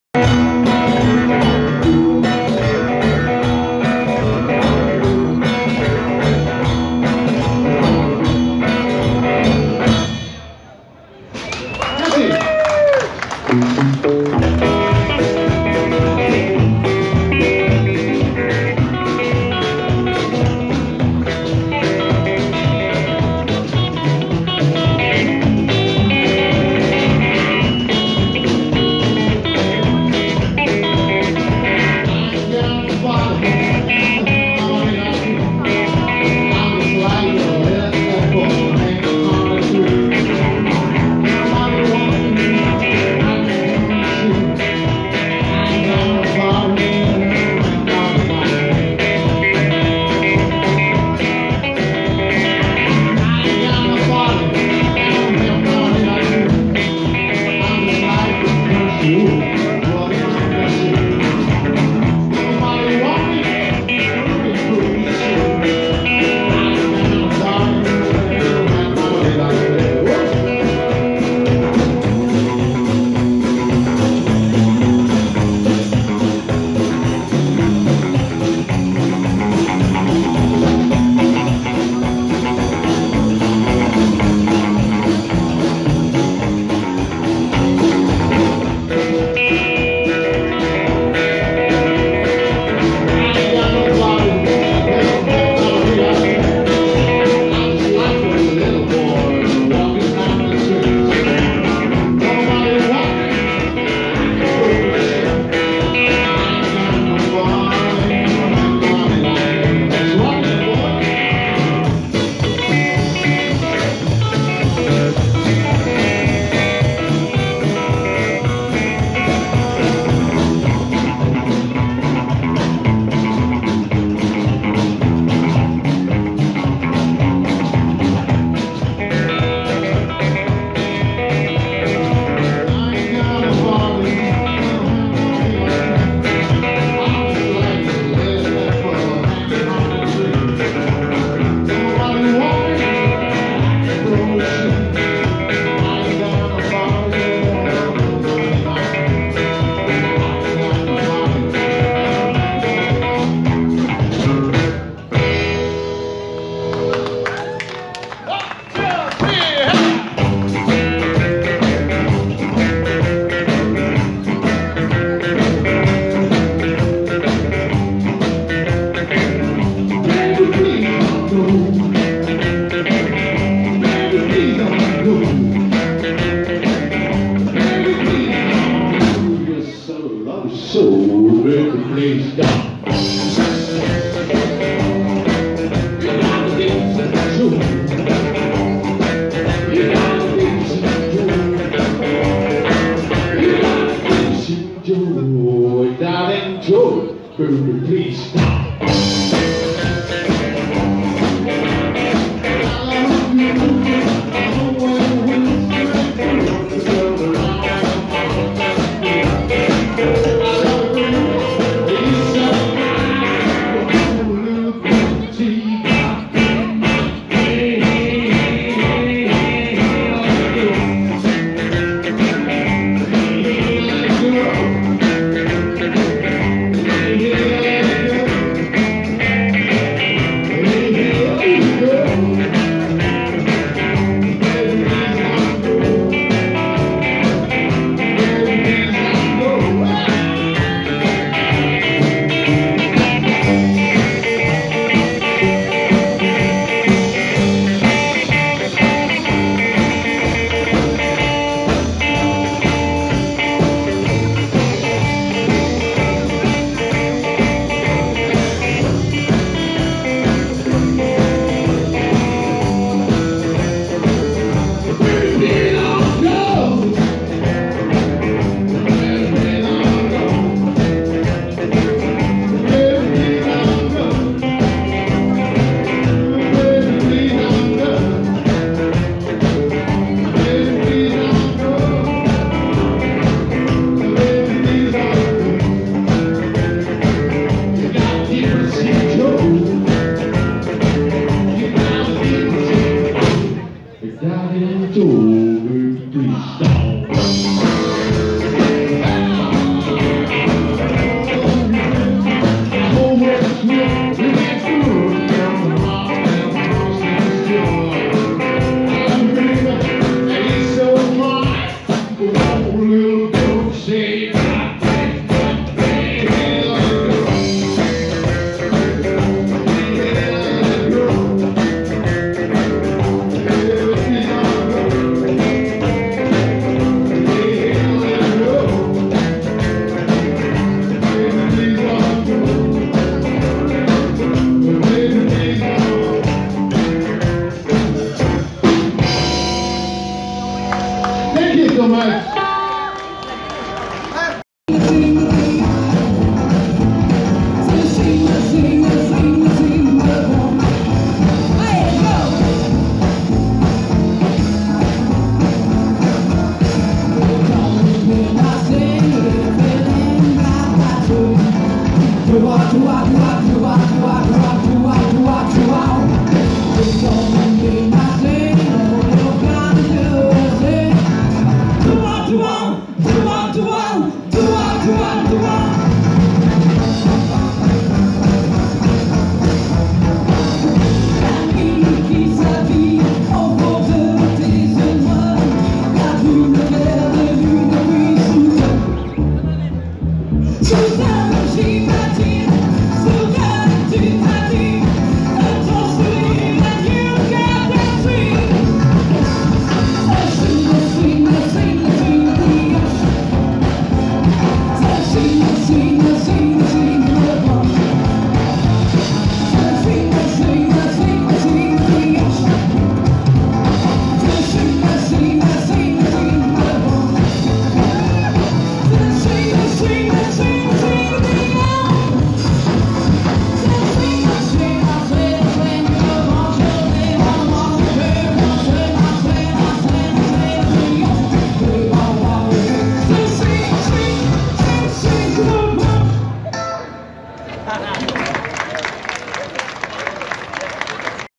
Béthune rétro 2019 - Mr Phil and the V eight Place du 73e
Ce week-end, Béthune rétro fête le rock, les baune rétro, c’est trois jours de rock’n’roll et de belles bagnoles, de pin-up bagnoles et les 20 ans des Hot Chickens
On ne change pas une équipe qui gagne. Béthune et de bananes, de robes à pois et de perfectos… bref, l’esprit de l’American way of life des années 1950. Cette 17e édition sera aussi l’occasion de fêter les 20 ans des Hot Chickens, chantres du rockab’ à la béthunoise. Tout est gratuit.
Ne venez pas à Béthune ce week-end si vous n’aimez pas le rock. Avec 42 concerts en trois jours, il y en aura pour tous les goûts, du rockabilly au doo-wop en passant par le rock’n’roll, le blues, la country, le surf, le garage…